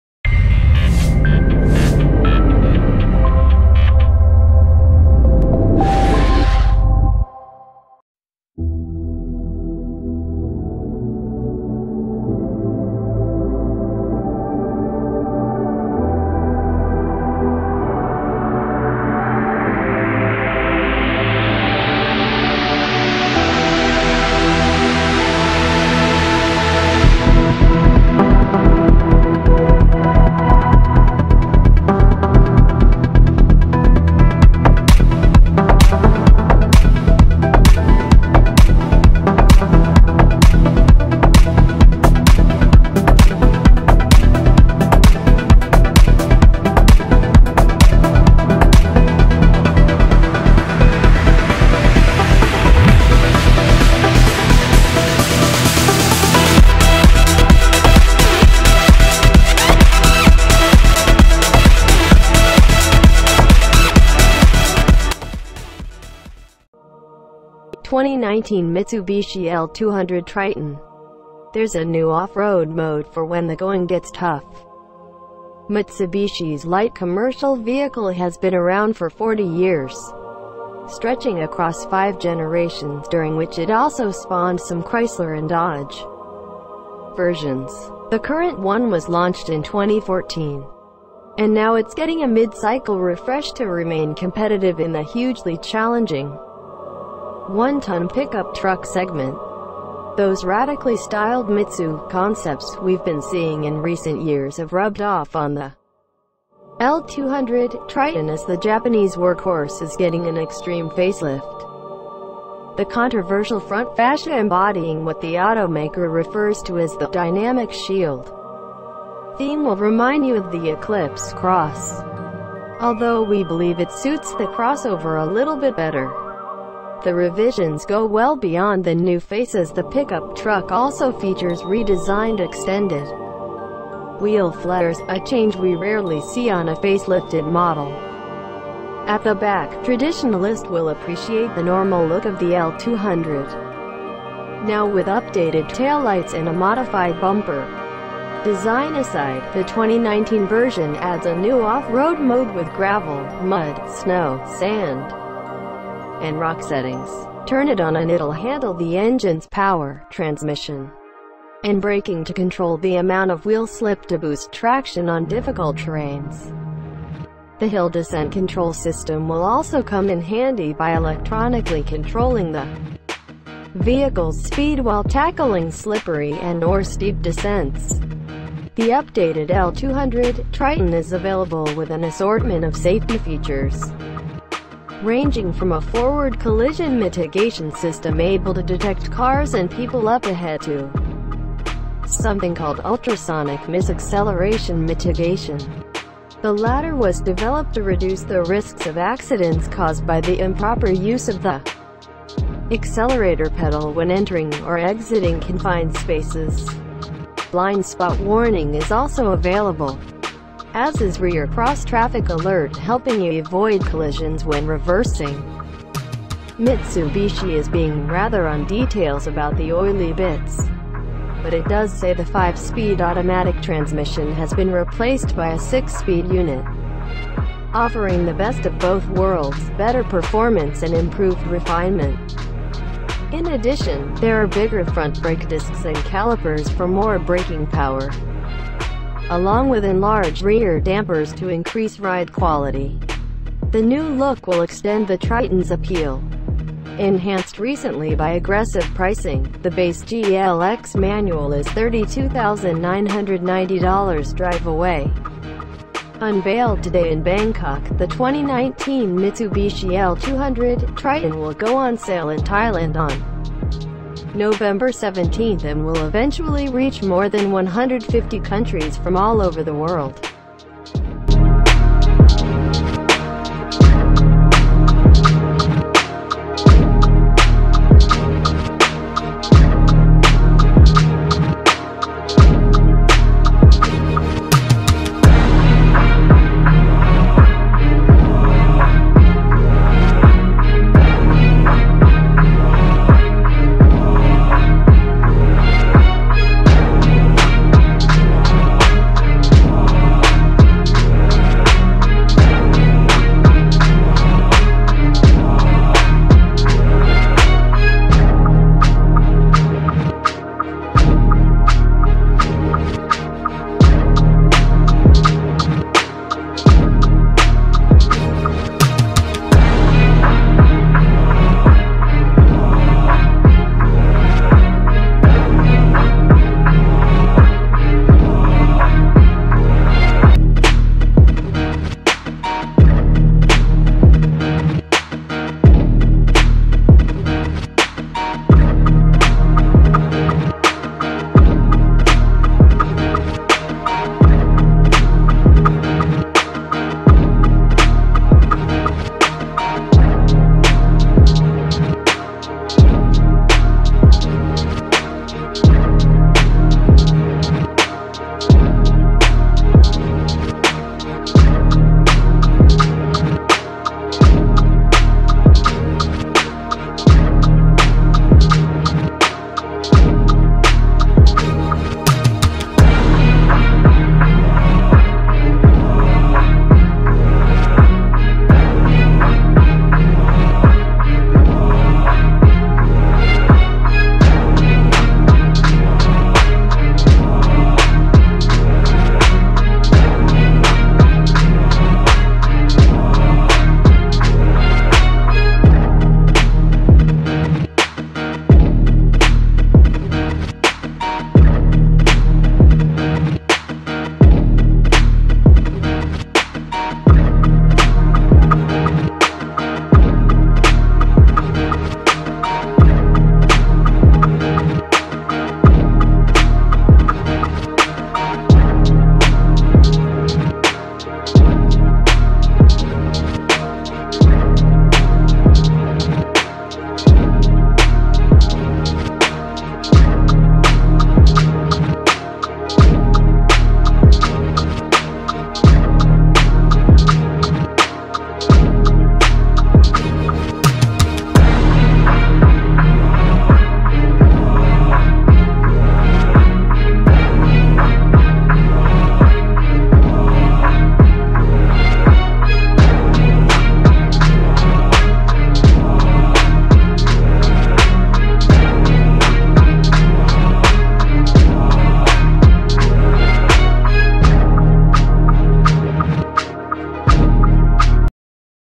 Mitsubishi L200 Triton 2019-the most powerful pickup climbing relentlessly
2019 Mitsubishi  L200 Triton
There's a new off-road mode for when the going gets tough.

Mitsubishi’s light commercial vehicle has been around for 40 years, stretching across five generations during which it also spawned some Chrysler and Dodge versions. The current one was launched in 2014, and now it’s getting a mid-cycle refresh to remain competitive in the hugely challenging one-ton pickup truck segment.

Those radically styled Mitsu concepts we’ve been seeing in recent years have rubbed off on the L200 / Triton as the Japanese workhorse is getting an extreme facelift. The controversial front fascia embodying what the automaker refers to as the “Dynamic Shield” theme will remind you of the Eclipse Cross, although we believe it suits the crossover a little bit better.

The revisions go well beyond the new face as the pickup truck also features redesigned extended wheel flares – a change we rarely see on a facelifted model. At the back, traditionalist will appreciate the normal look of the L200, now with updated taillights and a modified bumper.
Design aside, the 2019 version adds a new off-road mode with Gravel, Mud/Snow, Sand, and Rock settings. Turn it on and it’ll handle the engine’s power, transmission, and braking to control the amount of wheel slip to boost traction on difficult terrains. The Hill Descent Control system will also come in handy by electronically controlling the vehicle’s speed while tackling slippery and/or steep descents.

The updated L200 / Triton is available with an assortment of safety features, ranging from a Forward Collision Mitigation system able to detect cars and people up ahead to something called Ultrasonic Misacceleration Mitigation. The latter was developed to reduce the risks of accidents caused by the improper use of the accelerator pedal when entering or exiting confined spaces. Blind spot warning is also available, as is Rear Cross Traffic Alert helping you avoid collisions when reversing.
Mitsubishi is being rather on details about the oily bits, but it does say the five-speed automatic transmission has been replaced by a six-speed unit offering the best of both worlds: better performance and improved refinement. In addition, there are bigger front brake discs and calipers for more braking power, along with enlarged rear dampers to increase ride quality.

The new look will extend the Triton’s appeal, enhanced recently by aggressive pricing — the base GLX manual is $32,990 drive-away.

Unveiled today in Bangkok, the 2019 Mitsubishi L200 / Triton will go on sale in Thailand on November 17 and will eventually reach more than 150 countries from all over the world.